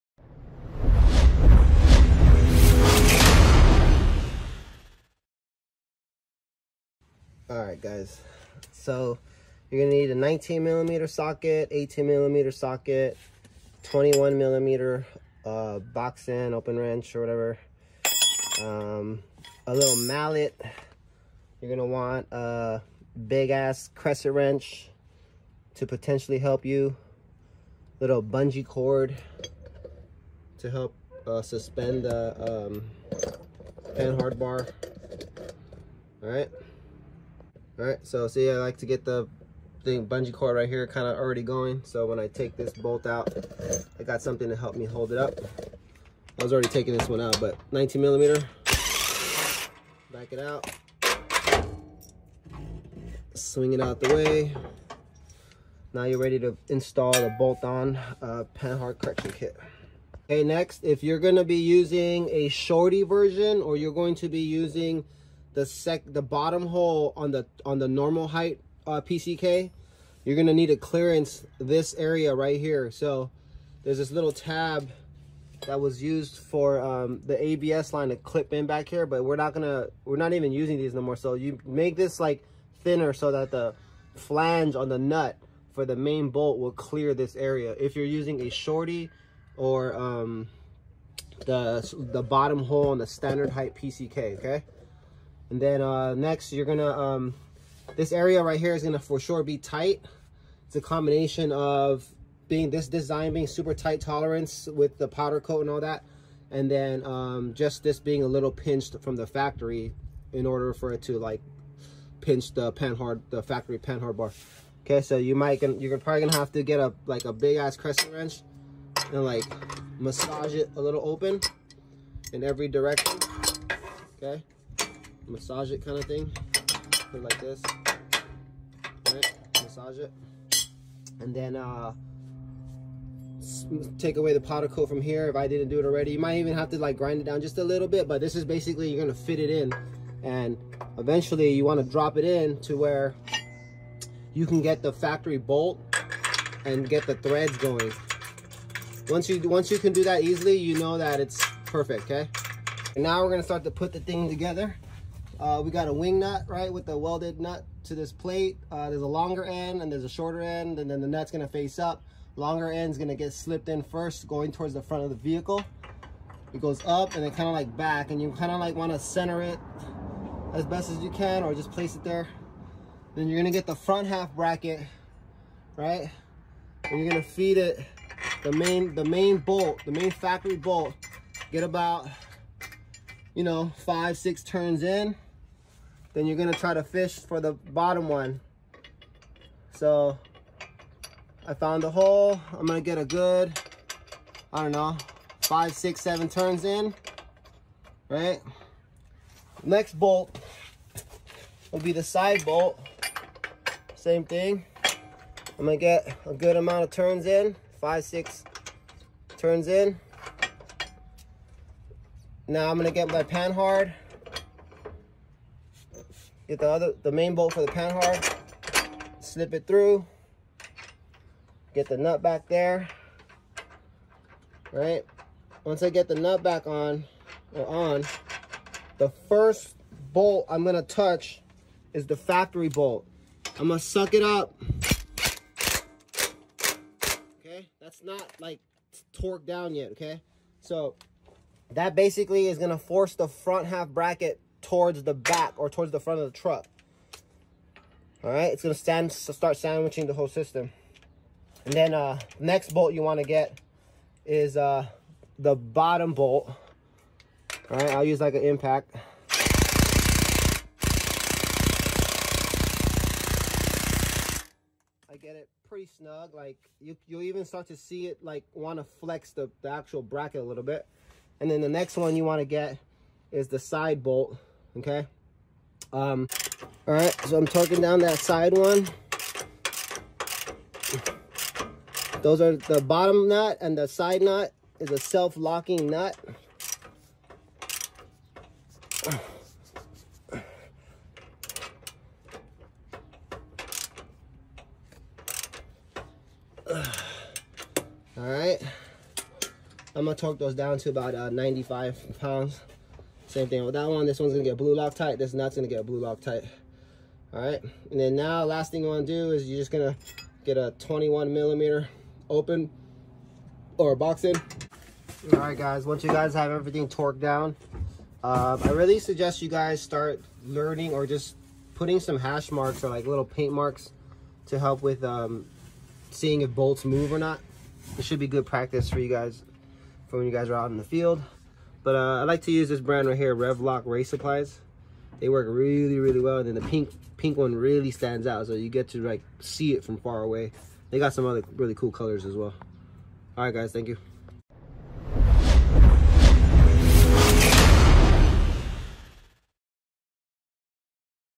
Install Video - KDSS BOLT ON PAN-HARD CORRECTION KIT
designed for 5th gen 4runner, 4th Gen 4runner , gx 460/470 , prado 150 and fj cruiser’s !!!

When you lift a solid axle that has a pan hard bar (aka track bar), depending on the lift height, the angle of that pan hard bar becomes so steep, that any natural cycling of the axle will cause a feeling of being unstable.  The reason for this is the pan hard bar being so angled, it forces the axle to swing left and right under the vehicle vs the axle cycling up and down.

To remedy this issue, you want to raise ( or lower frame side ) the pivot point on the axle so that the pan hard bar is parallel or as close to parallel to the ground / axle.   With the pan hard bar in this configuration , the natural cycling of up and down is returned ( for the most part , once you start to add a lot more travel ie long travel , things start to swing out again.   Regardless , this will still be beneficial to having vs not having ).

In this market, there has only really been one option for this Toyota / Lexus market.  It’s a weld on version. This new option opens up the market to allow one to easily install it themselves with simple common tools.  Not everyone has access to a welder or even access to some one that can weld.  With that said , you are still basically relying on the weld/ welder knowing what he’s doing by welding on a thicker piece of metal to the top of a thinner wall metal and In hopes of it not failing.  For the most part , it will not fail but All the load is being spread only along the weld.

This new bolt on pan-hard correction kit is designed with strength in mind.  The way it is designed, wrapped around , thick wing nut inside the oem bracket , the load is evenly distributed along the whole oem mount and not just the top where the welds are at.  On top of that you can easily toss in Some small spot welds if you are that guy that just needs it.  This thing is easily installed and or removed for whatever reason.  Just being bolted on and toque down properly, it is stronger then what’s on the market.

Now we add color options with high end powder coating from Ablaze Powdercoating here in norco ca.   You just can’t beat this option.

SHORTY OPTION:

the shorty version is for guys that need maximum up travel for high speed setup. Typically if you have some extended travel kings or fox shocks.  Those shocks have a little more travel but also still maintain oem bump travel.  If you want to maximize your shocks then you want the shorty.  Now if you have a really tall ride height (2.5” or taller ) then you ultimately need the regular height version as the very top 2nd hole is going to put your pan hard bar back into a more natural state ( parallel to the ground or close ).   To be honest , 99% of you guys will not contact it and will benefit from having the double hole ( normal height BOPCK) to allow adjustments if ride heights change due to load/ weight !!